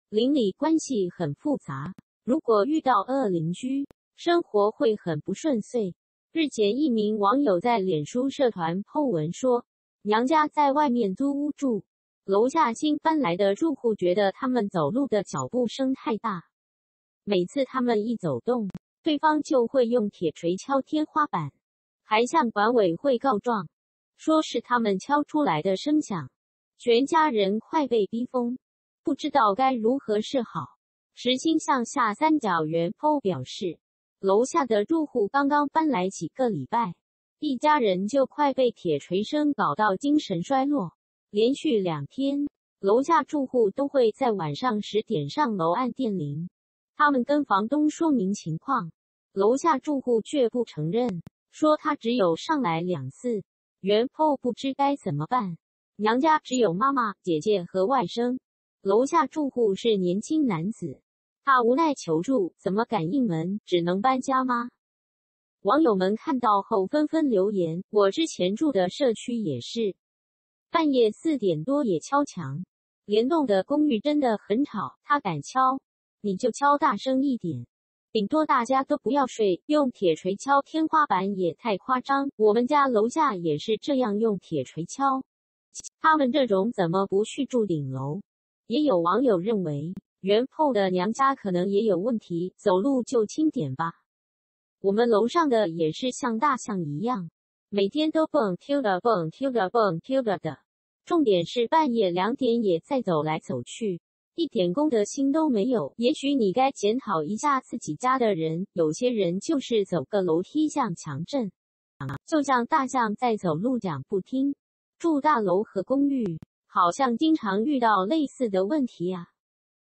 腳步聲被嫌太大!樓下鄰居秒「回敬鐵鎚」全家被逼瘋只能搬家嗎
鄰里關係很複雜，如果遇到惡鄰居，生活會很不順遂。日前一名網友在臉書社團po文說，娘家在外面租屋住，樓下新搬來的住戶覺得他們走路的腳步聲太大，每次他們一走動，對方就會用鐵鎚敲天花板，還向管委會告狀，說是他們敲出來的聲響。全家人快被逼瘋，不知道該如何是好。
▼原po表示，樓下的住戶剛剛搬來幾個禮拜，一家人就快被鐵鎚聲搞到精神衰落。連續2天，樓下住戶都會在晚上10點上樓按電鈴。他們跟房東說明情況，樓下住戶卻不承認，說他只有上來2次。原po不知該怎麼辦，娘家只有媽媽、姐姐和外甥，樓下住戶是年輕男子。她無奈求助：「怎麼敢應門？只能搬家嗎？」
網友們看到後紛紛留言：「我之前住的社區也是，半夜4點多也敲牆，連棟的公寓真的很吵」、「他敢敲，妳就敲大聲一點！頂多大家都不要睡」、「用鐵鎚敲天花板也太誇張」、「我們家樓下也是這樣用鐵鎚敲！他們這種怎麼不去住頂樓」。
也有網友認為，原po的娘家可能也有問題：「走路就輕點吧！我們樓上的也是像大象一樣，每天都蹦～蹦～蹦～的，重點是半夜2點也在走來走去，一點公德心都沒有」、「也許你該檢討一下自己家的人」、「有些人就是走個樓梯像強震，就像大象在走路講不聽」。住大樓和公寓，好像經常遇到類似的問題啊。